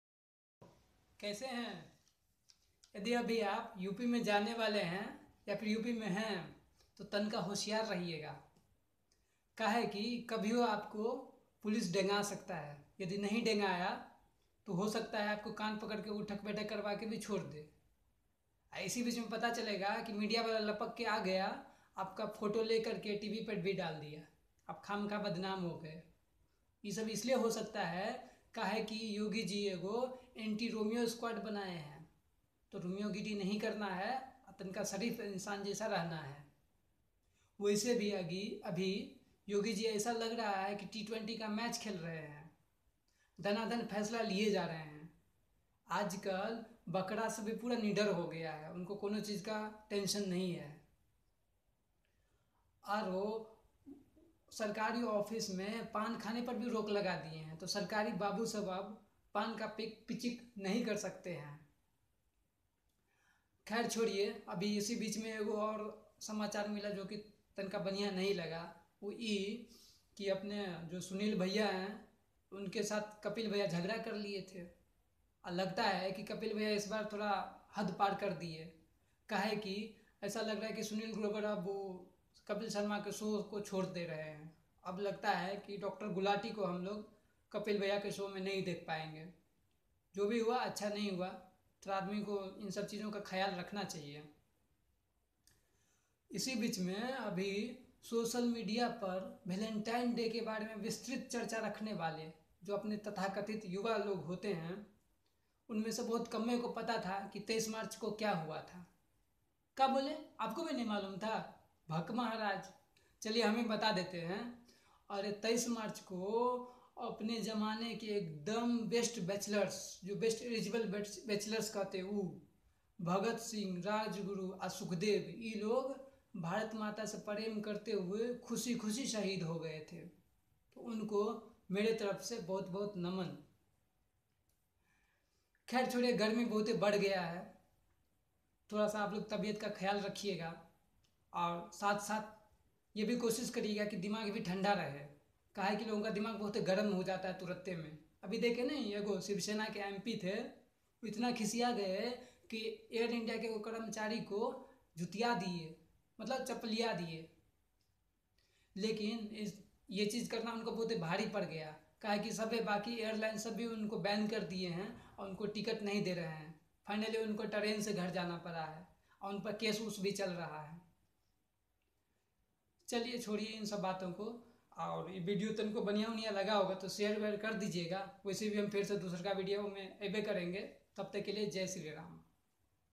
||Desi Talk with Gyan||Funny talk||Desi news||Bihari Comedy Talk||humoristic talk of the week||
New way of comedy with information in desi style, humoristic talk of the week, just smile.

This video gives you the glimpse of some interesting news of the week in completely desi and funny style.